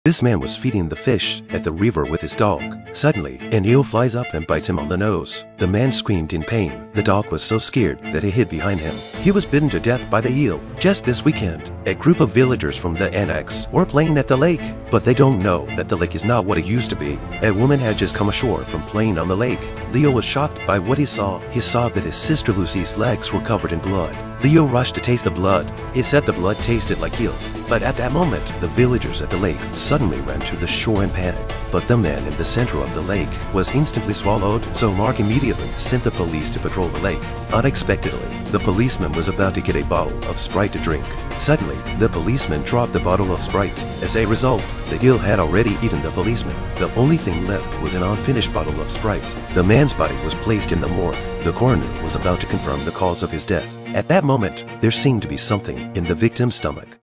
Man-eating eels have appeared in the town.1/2
The story takes place in a small town in Michigan, where the dreaded killer seven-gill eels are terrorizing the local residents. Whether you're swimming in the pool or sitting on the toilet, the killer eels can appear at any time and cost you your life. Seven-gill eels are eels with giant teeth, funnel-shaped mouths, and long, slender bodies that can travel freely through city sewers. When they go on a killing spree, no matter where you hide, you won't be safe. Who will be the next victim?

(Take you to understand a different movie story
Focus on film and television commentary
Moe new author, thank you all for your support~
If you like it, click a subscription